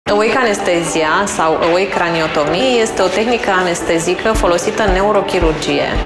AWAKE ANESTHESIA – Dr. Diana Toma, medic primar ATI MedLife
„Awake anesthesia” sau „awake craniotomy” este o tehnică anestezică folosită în neurochirurgie atunci când leziunea, tumorală sau vasculară, este localizată într-o arie corticală cu importanță funcțională – arie motorie, arie vizuală sau de vorbire. Avantajul acestei tehnici constă în rezecția maximală a leziunii cu minim de sechele neurologice.